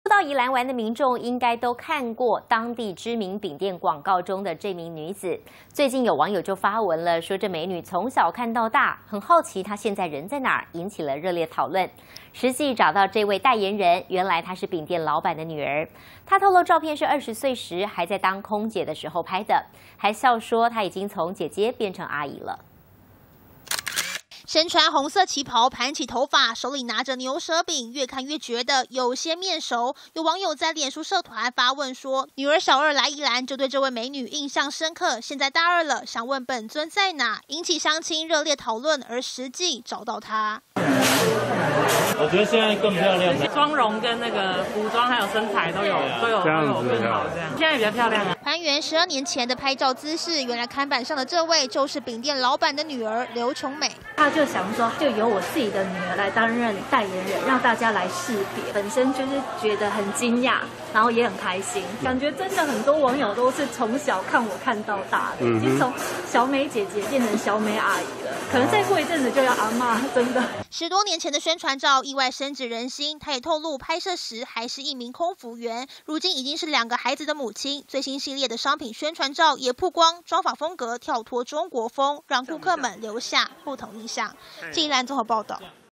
宜蘭人共同記憶! 餅舖看板美人身分曝光
許多到宜蘭玩的民眾，應該都看過、當地知名餅店廣告中的這位女子。最近還有網友發文，這美女從小看到大，好奇她現在人在哪，引起熱烈討論。實際找到這位代言人，原來她是餅店老闆的女兒。她透露，照片是２０歲、還在當空姐時拍的，還笑說「已經從姐姐變成阿姨了。」